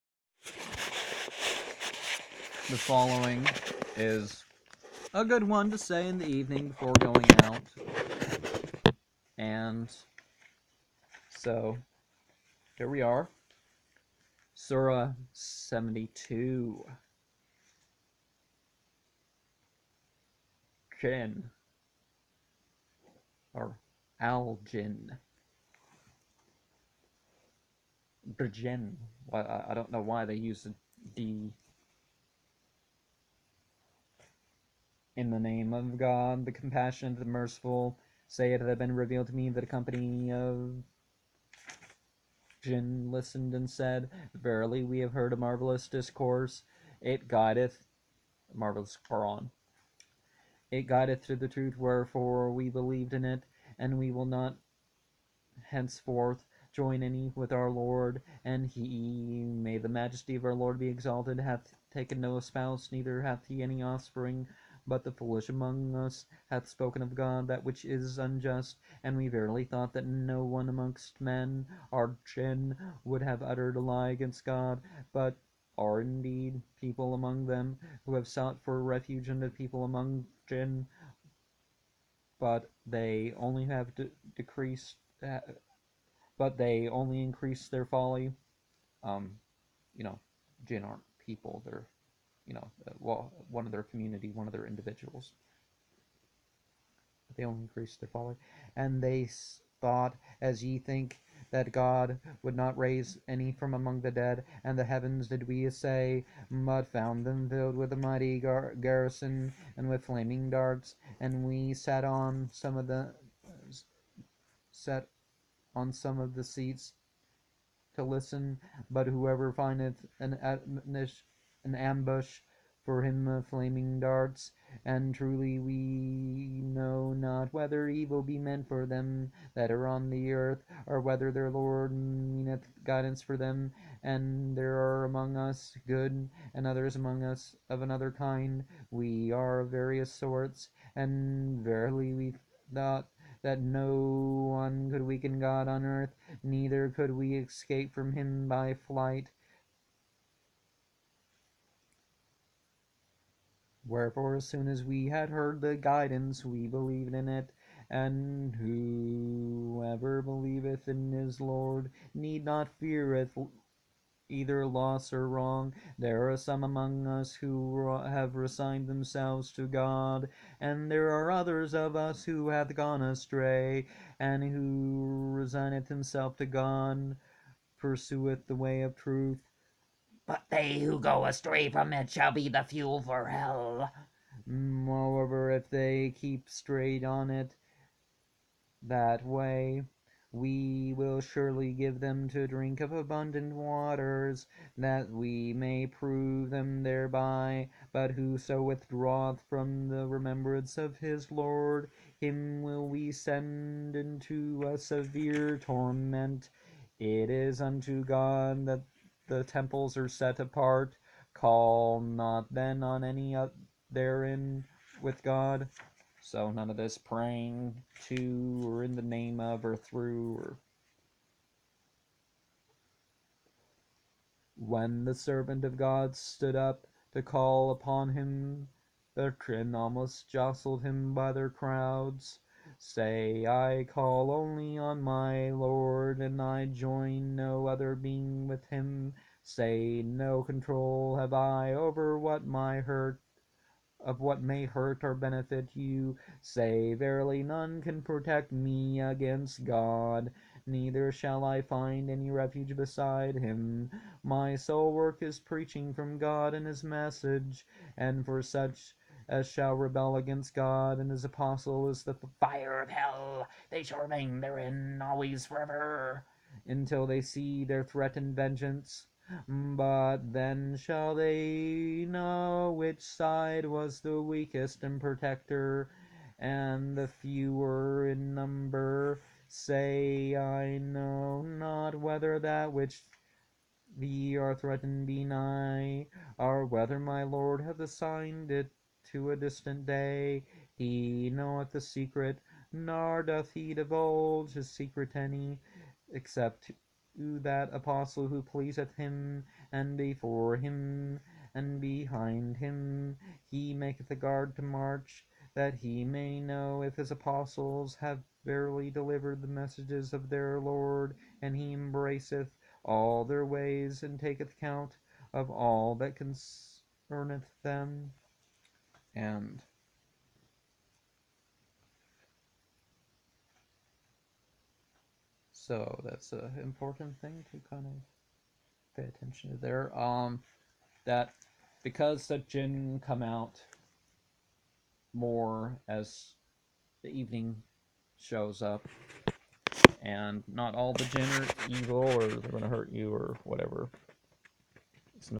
Surah 72 Djinn or Al Jinn of The Koran Translated by J.M. Rodwell 1861
Surah 72 Djinn or Al Jinn of The Koran Translated by J.M. Rodwell 1861  reminds us what spiritual centers are for and that all beings of choice may be guided to the right religion.